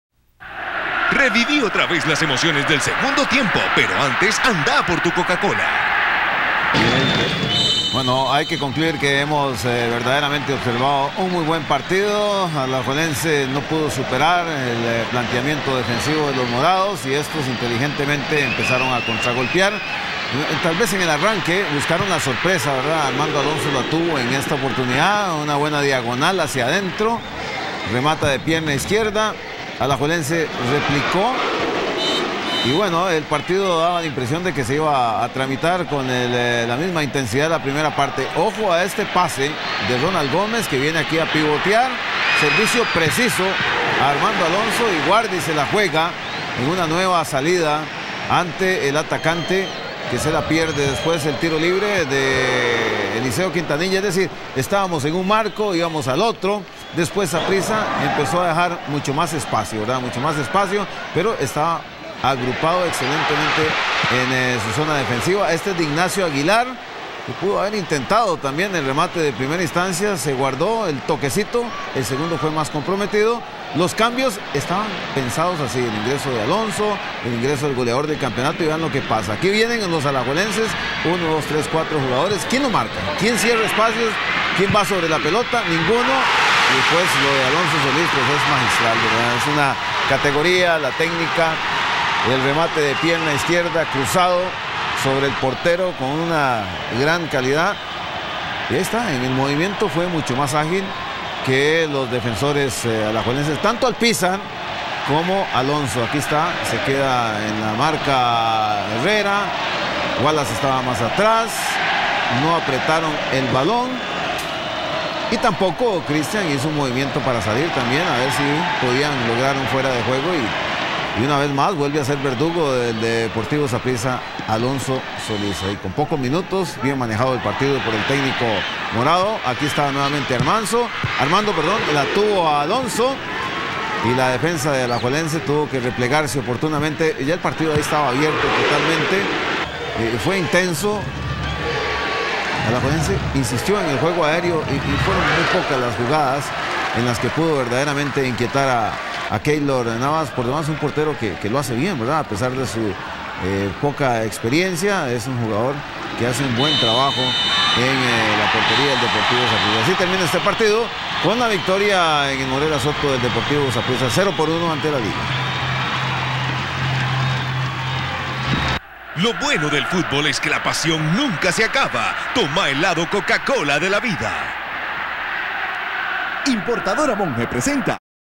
LIGA - SAPRISSA / 16-MARZO-2008 / 0-1
Como dijo Vergara, ya la LIGA no es rival, ES CLIENTE!!! Nuevamente Saprissa vuelve a ganarle a la Liga en el Morera Soto con golalzo de Alonso Solis, partido con el que ya la S lleva (a la fecha) 17 encuentros sin perder ante el equipo (¿?) rojinegro, lo que los morados consideramos ya como un abuso (o no, manudos?). Saludos al Mariachi, y muchos exitos con el NECAXA. Como diría Daddy Yankee: "Who's Daddy?" "The Big Boss"